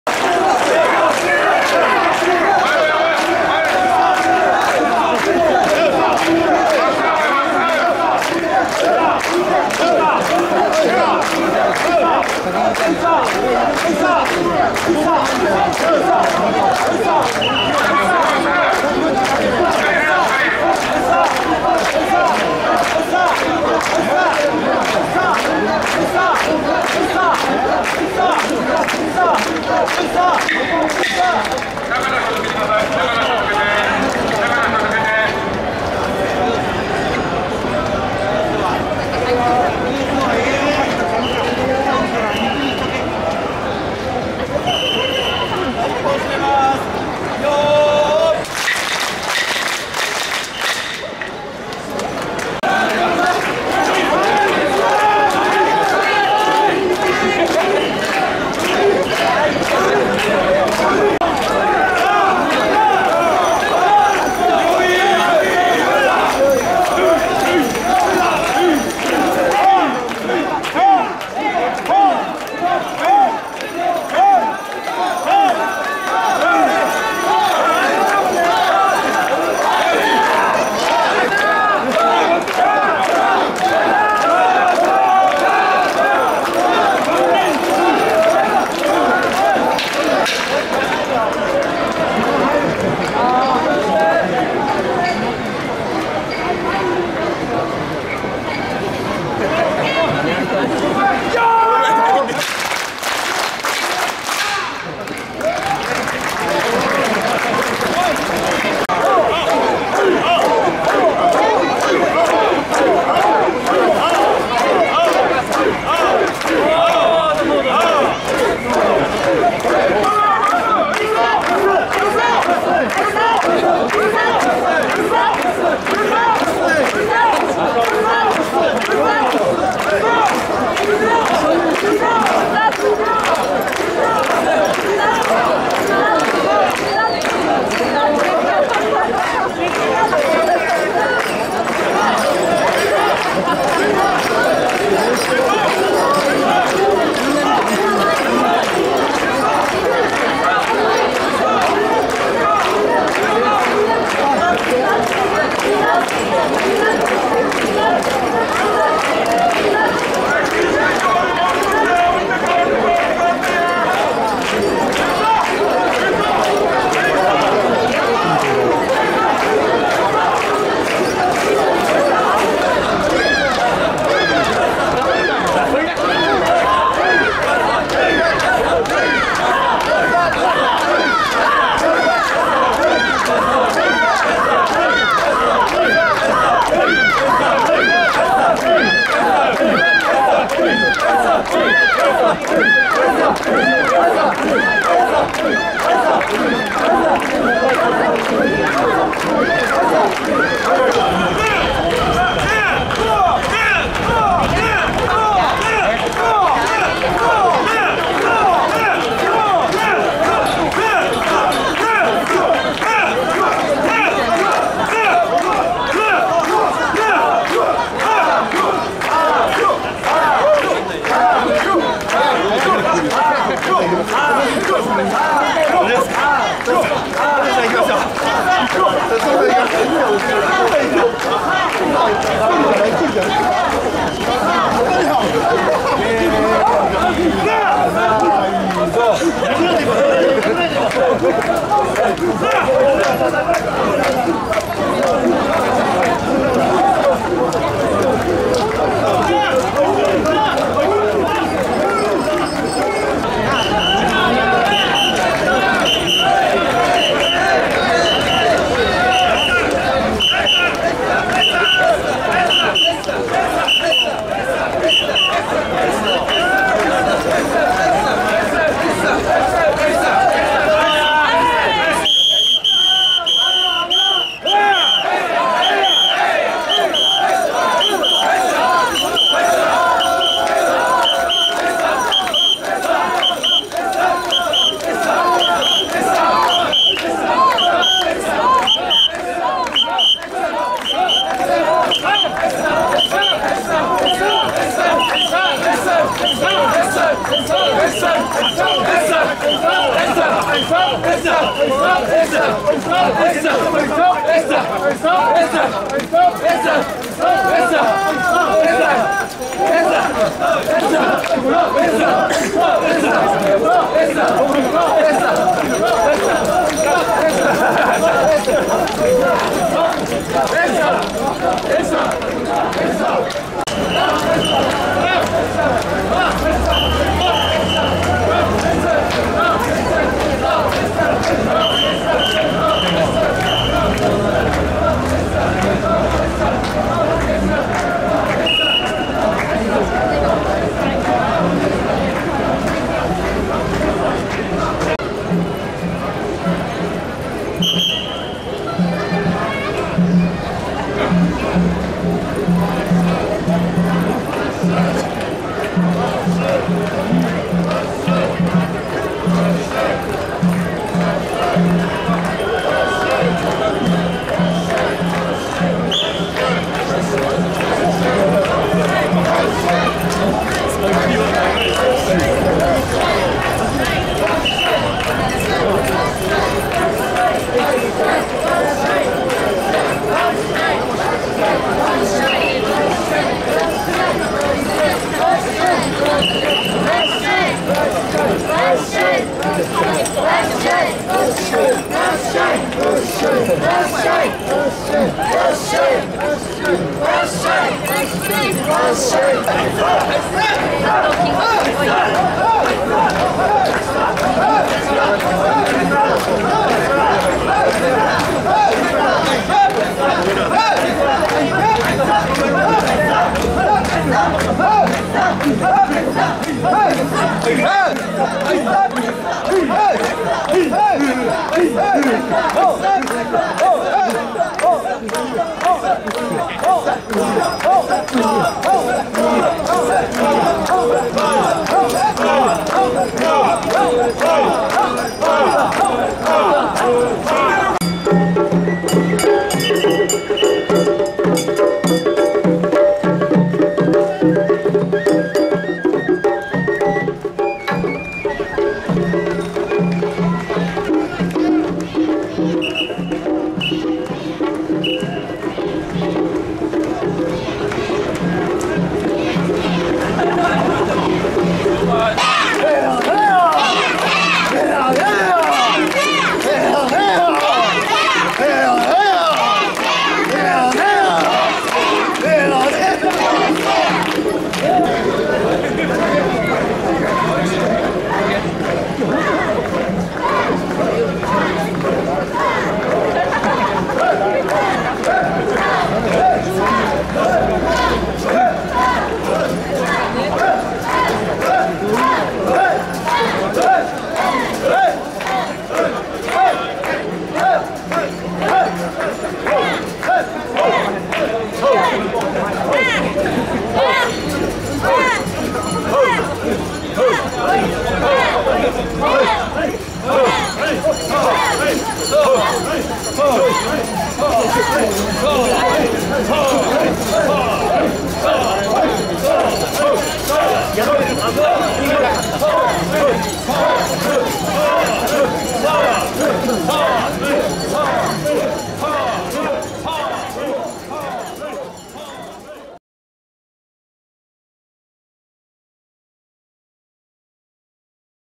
【神田祭】【神輿】【淡路町】4K 神田祭の神輿は大人気。淡路町に続々と集合する町内の神輿の大集合。神輿をかつぎ練り歩きながら盛り上がり、休憩の前の手締めも見どころです。
神田祭の神輿は大人気。淡路町に続々と集合する町内の神輿の大集合。神輿をかつぎ練り歩きながら盛り上がり、休憩の前の手締めも見どころです。

コメントも歓迎します。